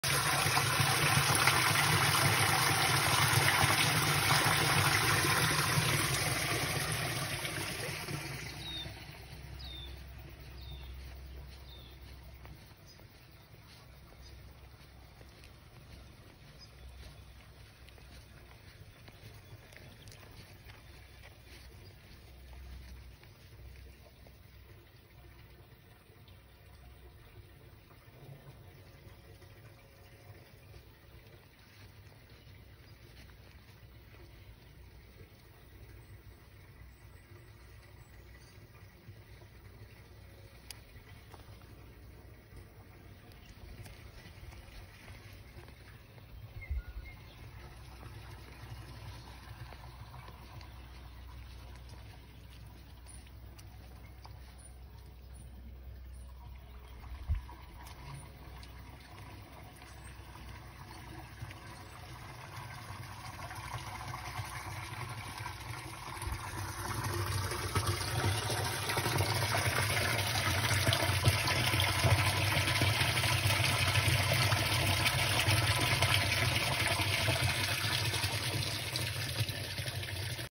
Fast running water leak outside 87 Volga Street, Island Bay
This has had many reports. I reported it a month ago. Still running and much worse now, including damage to the road.